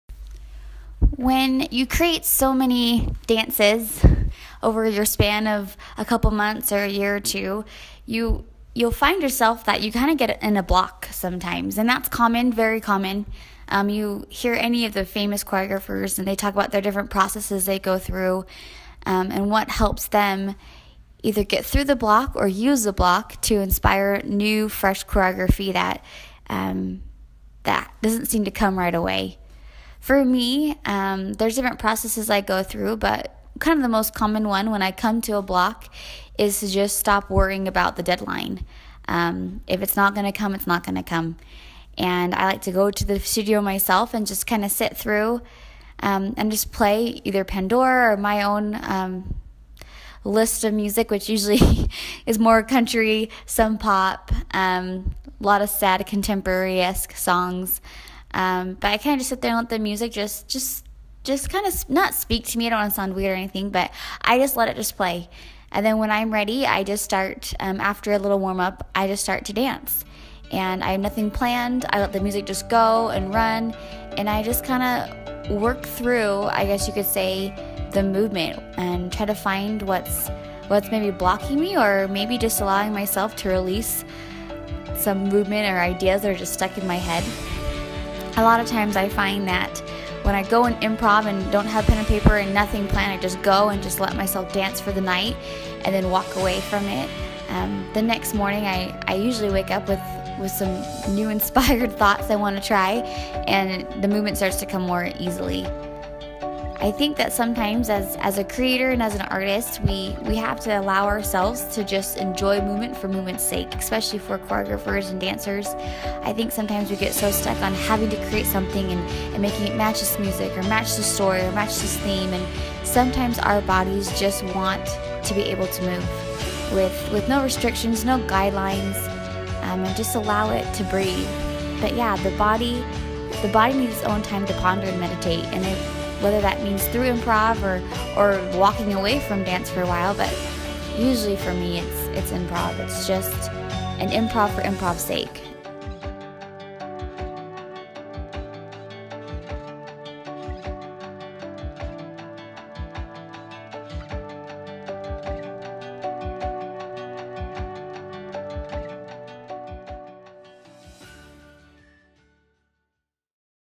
Improv | Shay Lynn Choreography
Decided to hit the studio for a little improv session, and thought why not share it with you! One of my favorite things to do when I need a break from the deadlines

Videography: Mckell Bundy
Editing & Sound: Shay Lynn